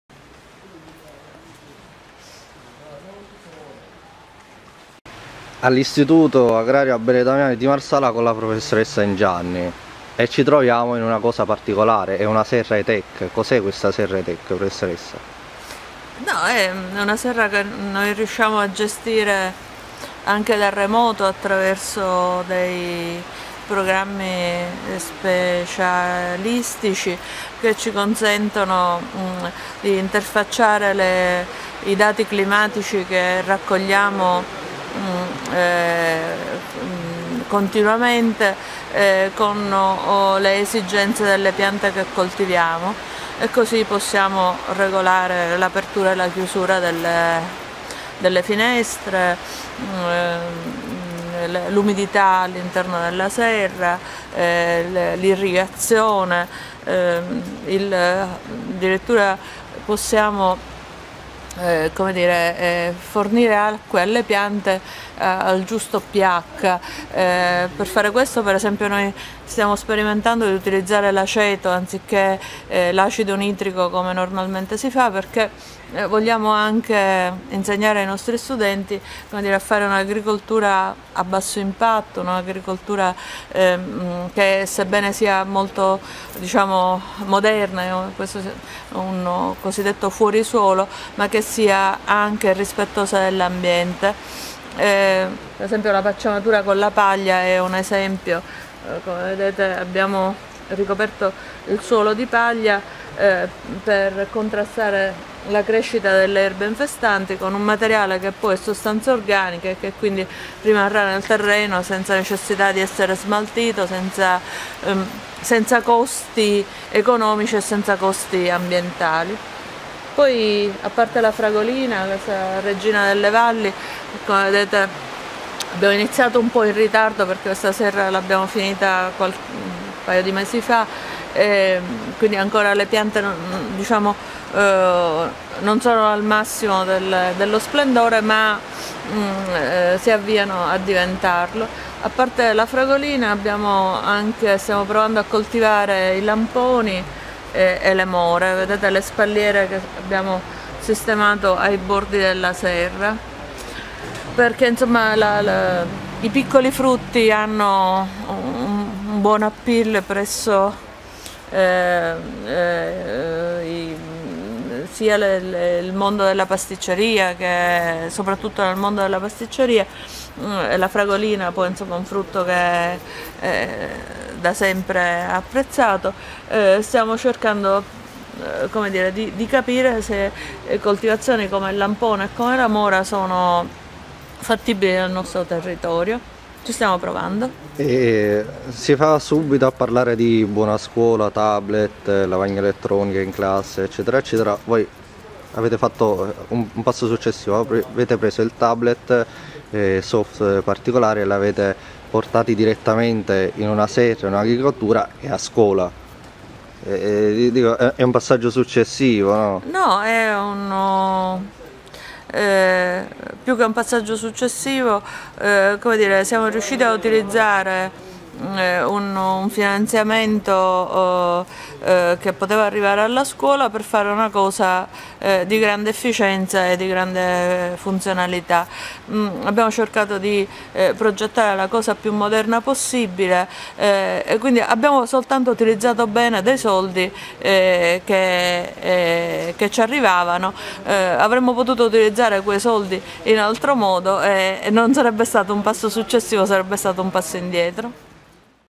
All'Agrario di Marsala si studia l'agricoltura 2.0 con la serra hi-tech
Un software per tenere sotto monitoraggio le coltivazioni, per dare acqua alle piante anche a distanza, con una stazione meteo che in tempo reale fornisce dati su temperatura, tasso di umidità.
E’ l’agricoltura 2.0 che si studia all’istituto Agrario Abele Damiani di Marsala con la serra Hi-tech. Un serra a tutti gli effetti, installata ormai da un paio di mesi all’interno dell’istituto marsalese che offre agli studenti di studiare sul campo, nel vero senso della parola, l’agricoltura sostenibile. Anche con l’utilizzo della tecnologia. La particolarità della serra hi-tech, infatti, è quella di avere una stazione meteo che in tempo reale fornisce tutti i dati utili per la coltivazione e il trattamento delle piante.